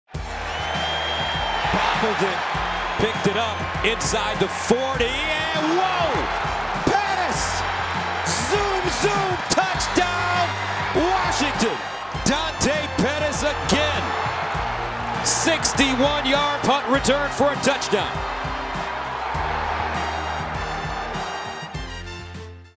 Highlight: Washington's Dante Pettis ties Pac-12 record with 6th career punt return touchdown
Washington receiver Dante Pettis streaks past the Rutgers defense en route to a 61-yard punt return touchdown in Friday night's 30-14 win in Piscataway Township, New Jersey. The touchdown ties Pettis with Cal alum DeSean Jackson for the most career punt return touchdowns in Pac-12 history (6).